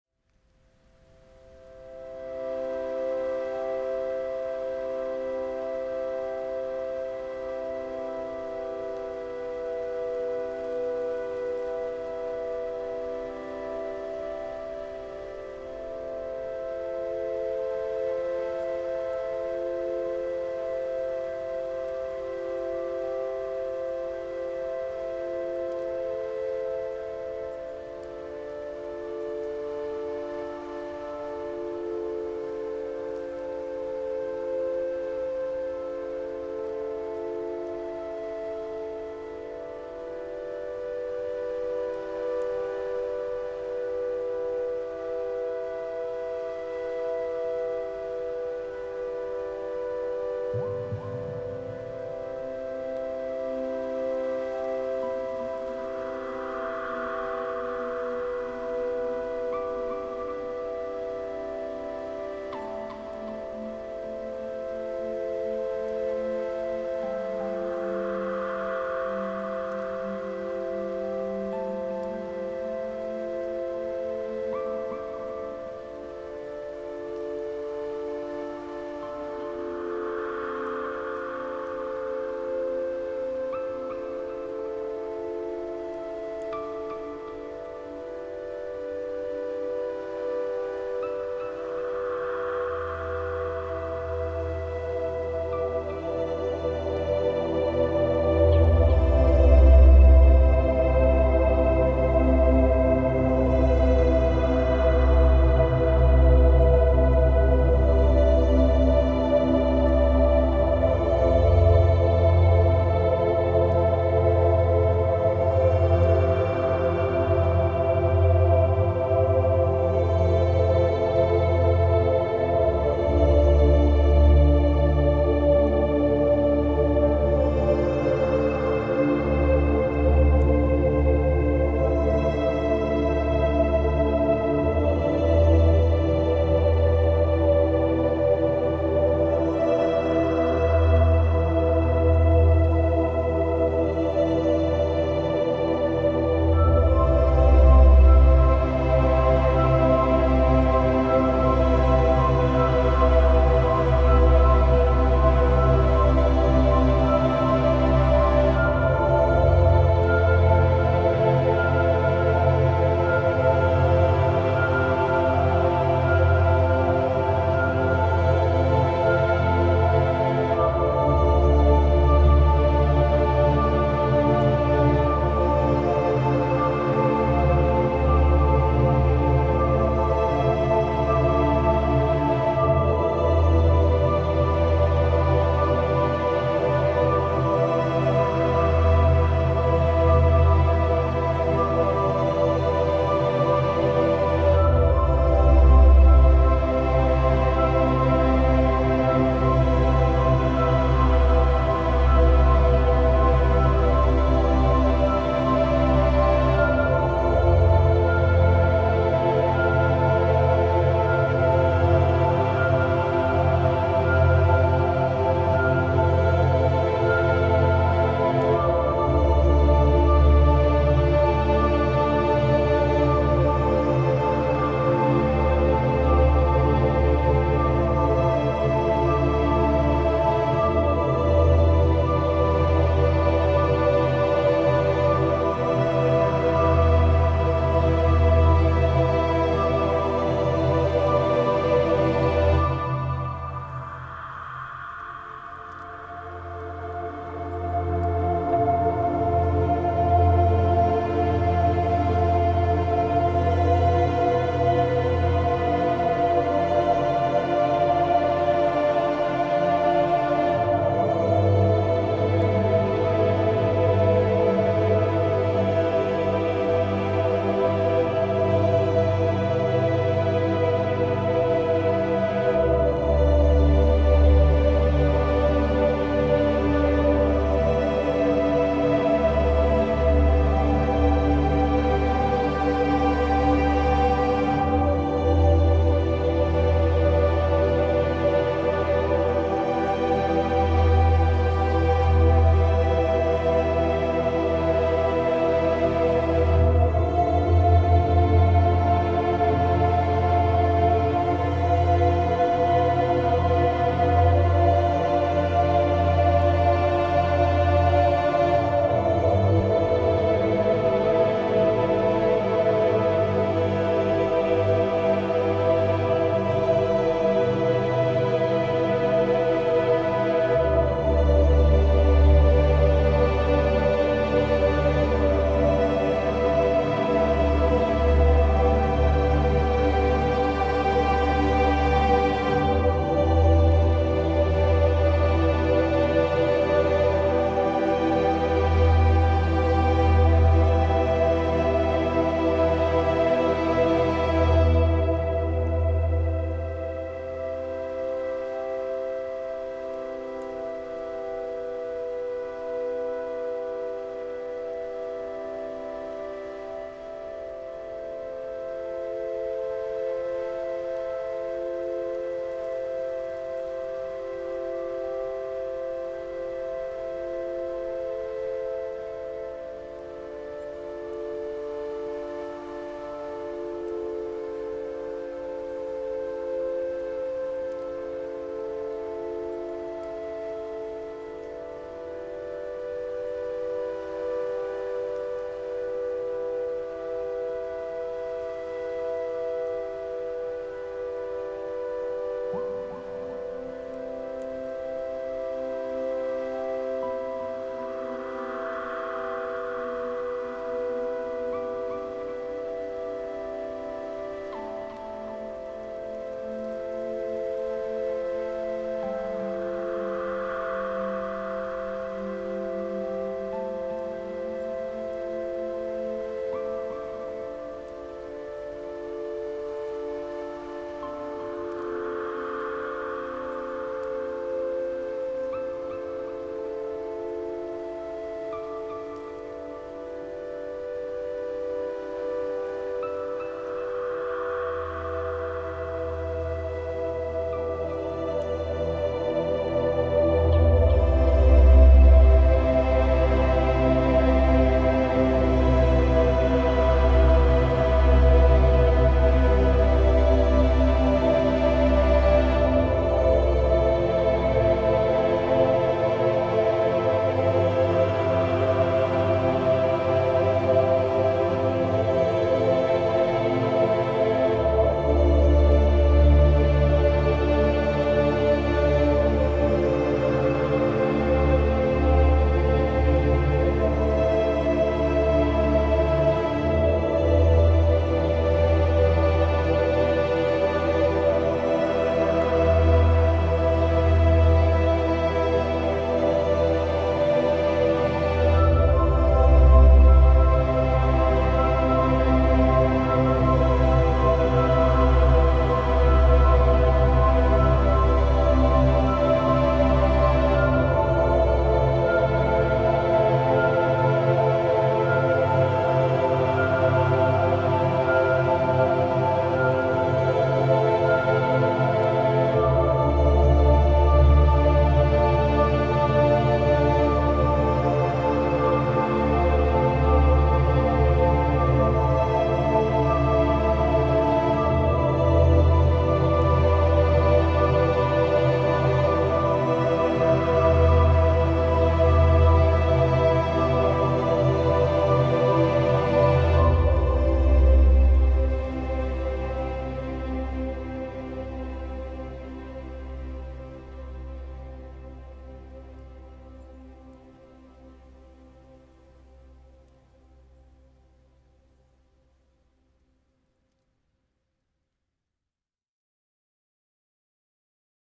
Relaxing music,Nature music,Sleep music,Meditation music,Calming music...
A gentle relaxing music,useful to reduce stress,to sleep and also to meditate